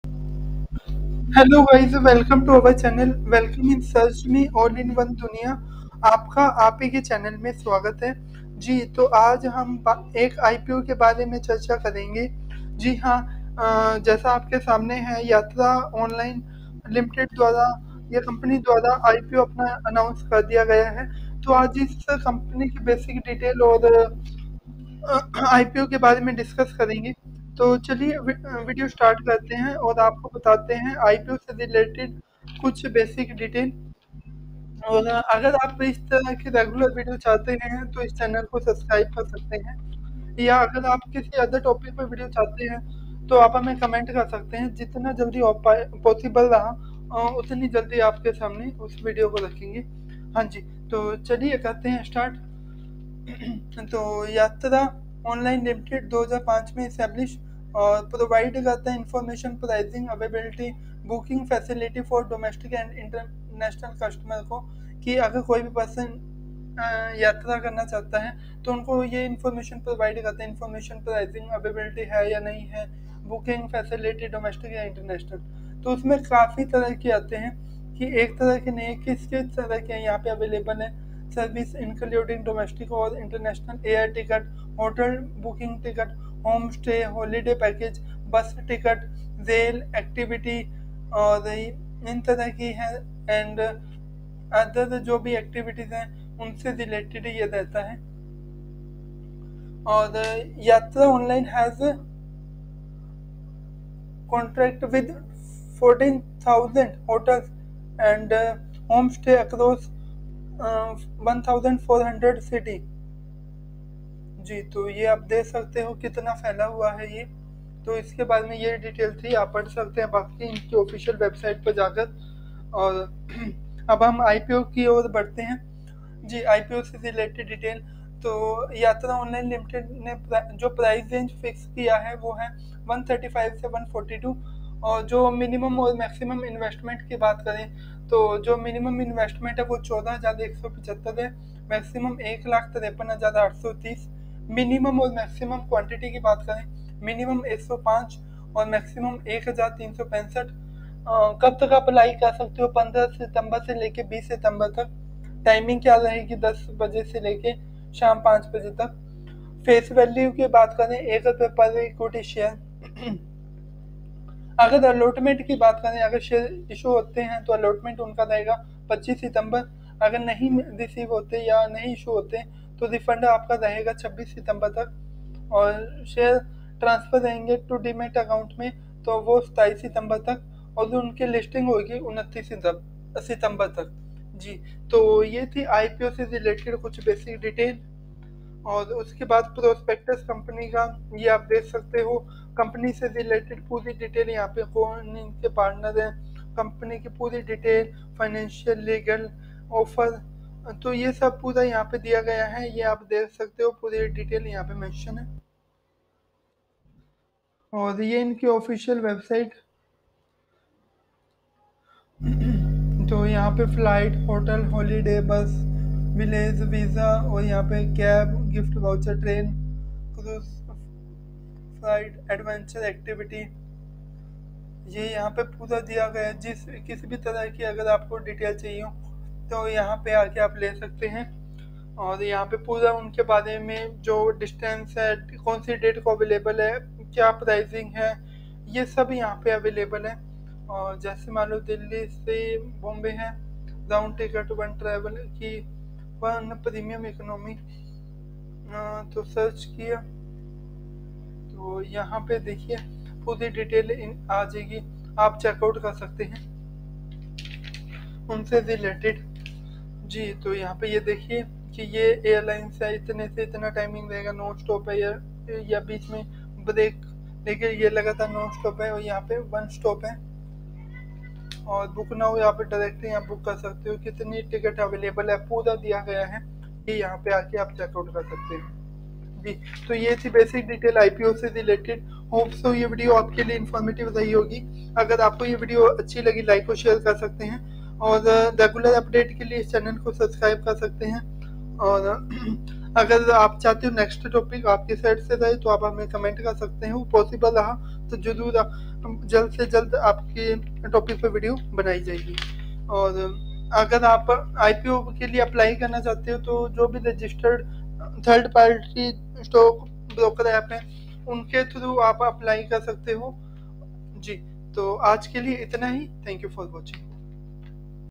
Yatra Online Limited (YOL) - SME IPO full details 2023-----SearchMe (All-in-one-dunia)
यात्रा ऑनलाइन लिमिटेड- एसएमई आईपीओ एसएमई आईपीओ पूर्ण विवरण: जीएमपी, सदस्यता स्थिति, आवेदन तिथि, समय, निवेश और अन्य विवरण (Yatra Online Limited - SME IPO Full Details: GMP, subscription status, Apply date, Timing,Investment and other details)